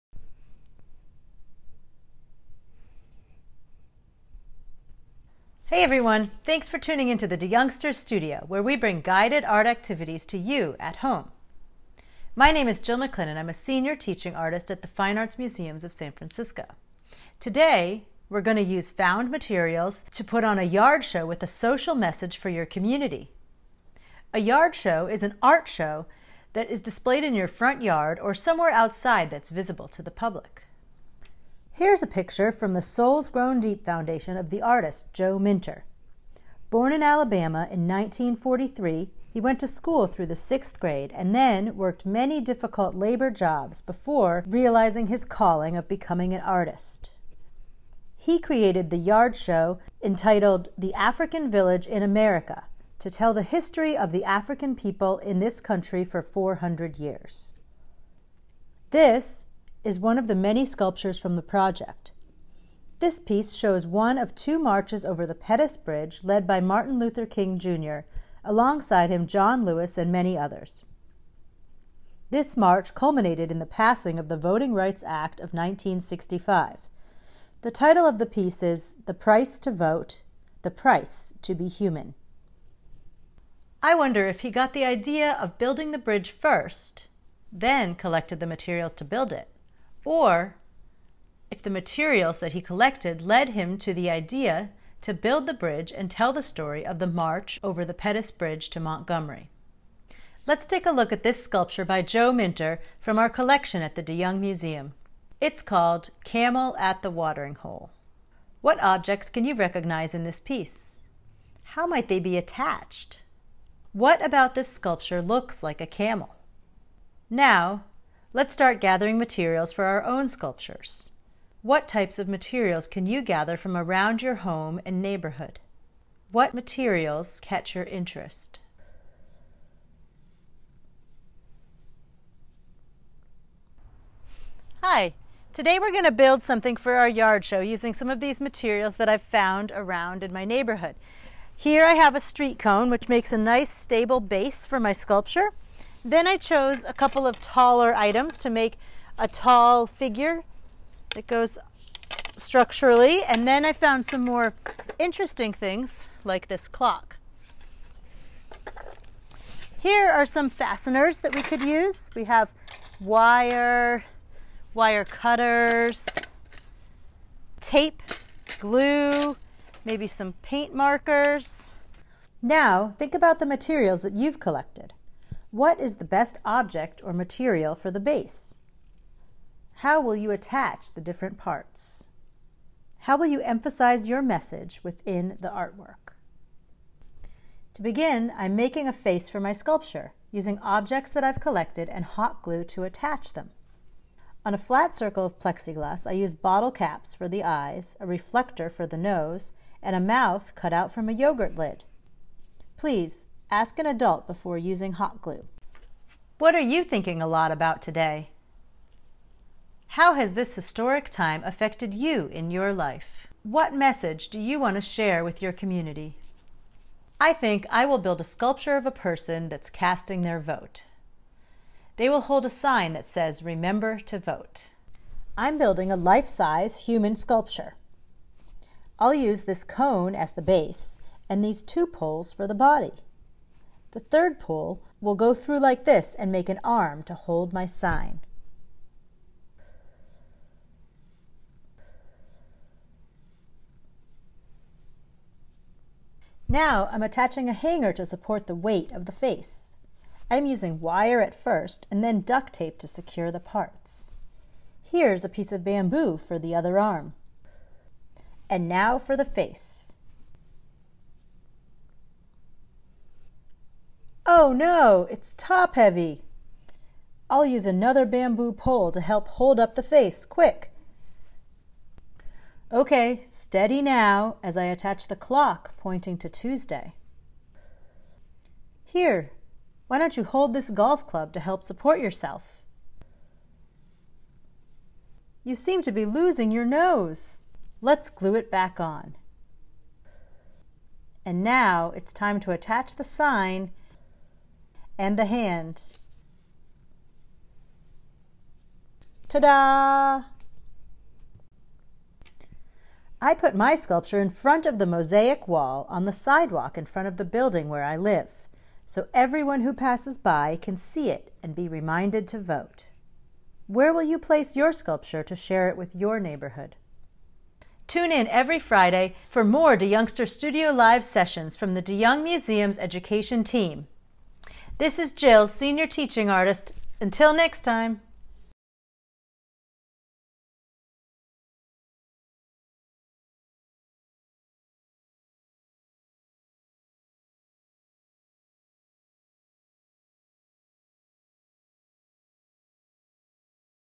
How To: Yard Show Art Making Inspired by Joe Minter
Jill McLennan, Senior Teaching Artist, leads us in this landscape painting lesson that includes six easy steps. See here for the lesson plan: